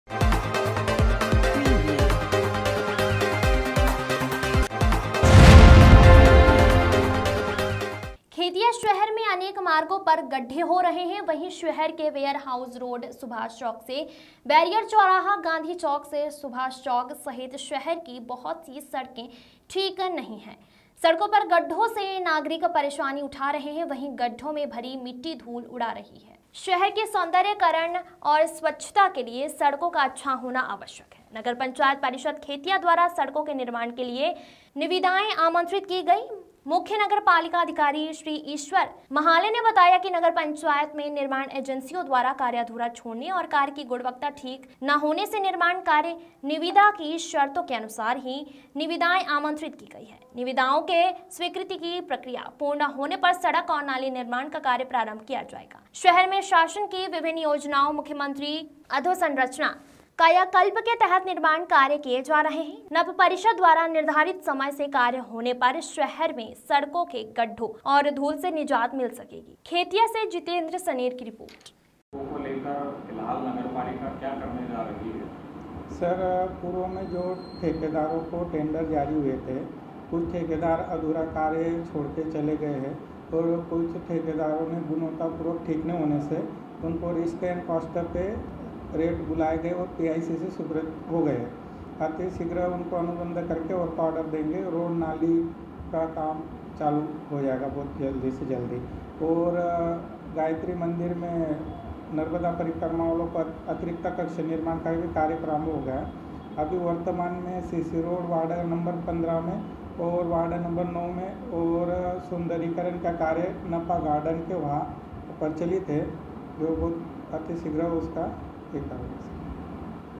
खेतिया - सड़क निर्माण के लिए निविदाएं आमंत्रित, सौदर्यीकरण के लिए हुई स्वीकृति
सड़क निर्माण के लिए निविदाएं आमंत्रित
सौदर्यीकरण के लिए हुई स्वीकृति